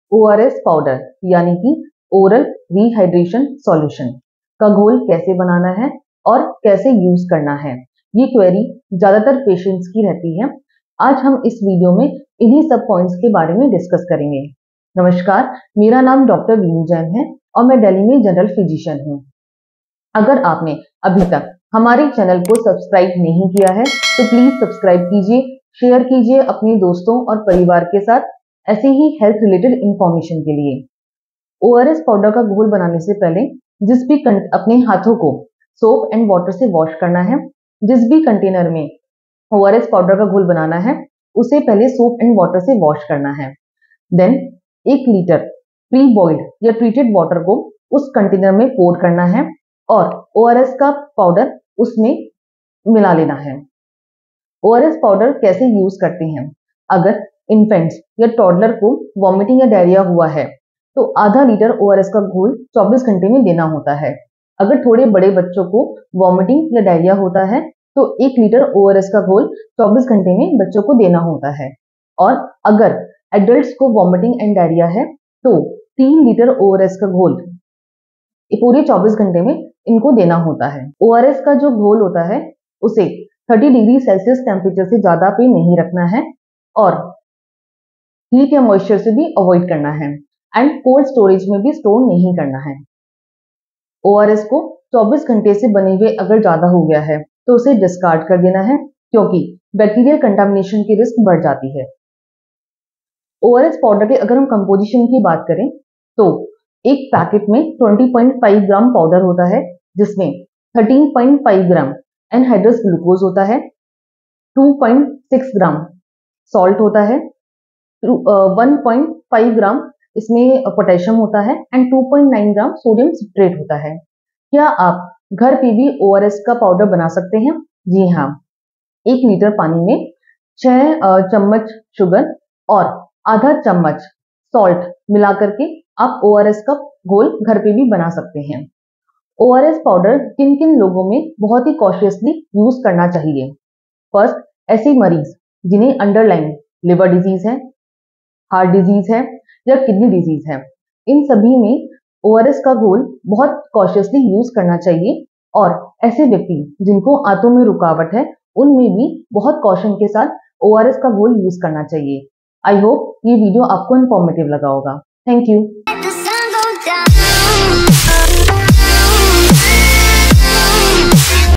ORS Powder को घर पर कैसे बनाएं | How to Make ORS Powder at Home | Dr Veenu Jain | MN Medicare Center
नमस्कार दोस्तों! इस वीडियो में हम आपको दिखाएंगे कि ORS (ओरल रिहाइड्रेशन सॉल्ट) पाउडर को घर पर आसानी से कैसे बनाएं। ORS डिहाइड्रेशन से लड़ने में मदद करता है और इसे घर पर बनाना बहुत ही सरल है। इस वीडियो में, हम आपको स्टेप बाय स्टेप गाइड देंगे ताकि आप भी जरूरत पड़ने पर ORS पाउडर बना सकें। हमें उम्मीद है कि यह वीडियो आपके लिए उपयोगी साबित होगा।

Hello friends! In this video, we will show you how to easily make ORS (Oral Rehydration Salt) powder at home. ORS helps combat dehydration and is very simple to make at home. In this video, we will provide you with a step-by-step guide so you can make ORS powder whenever needed. We hope this video will be useful for you.